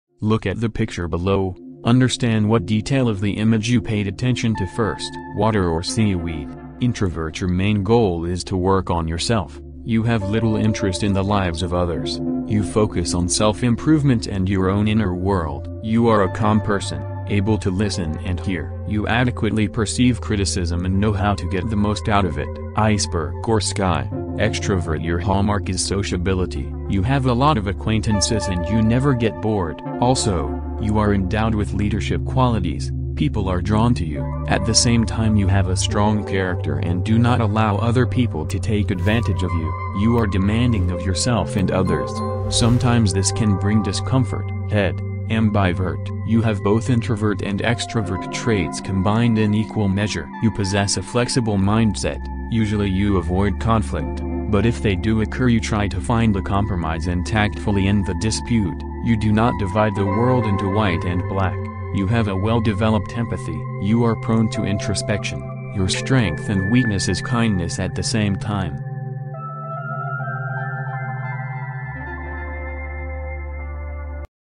What you see first will tell you whether you are an extrovert or an introvert. Psychological test.
Psychological test. Psychology.